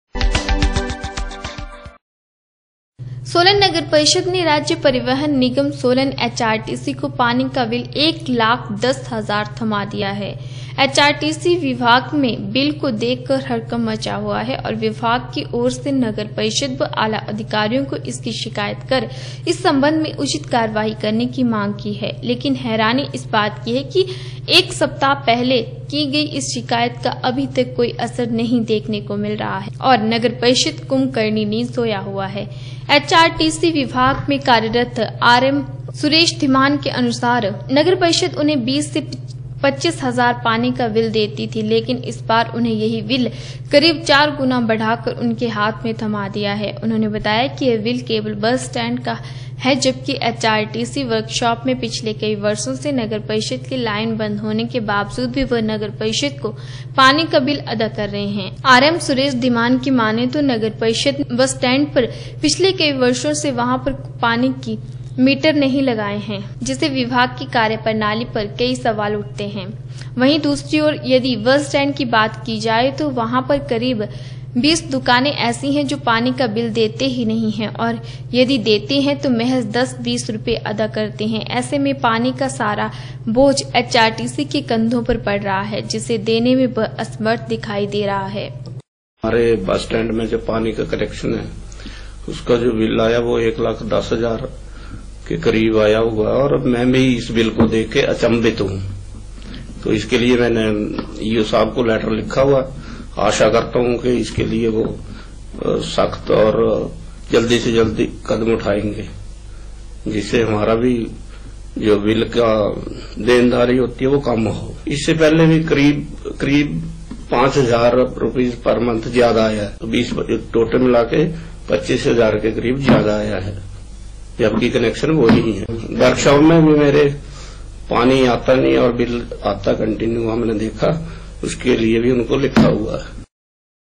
सोलन नगर परिषद ने राज्य परिवहन निगम सोलन एच आर टी सी को पानी का बिल एक लाख दस हजार थमाया
सोलन नगर परिषद ने राज्य परिवहन निगम सोलन एच आर टी सी को पानी का बिल  एक लाख दस हजार थमा दिया है | एच आर टी सी विभाग  में बिल को देख कर  हड़कंप मचा हुआ है और विभाग की ओर से नगर परिषद  व्  आला अधिकारीयों को इसकी शिकायत कर इस संबन्ध  में उचित कार्यवाही करने की भी मांग की है | लेकिन हैरानी इस बात की है कि एक सप्ताह पहले की गई इस शिकायत का अभी तक कोई असर नहीं दिख रहा है | और नगर परिषद कुम्भकर्णी  नींद सोया हुआ है |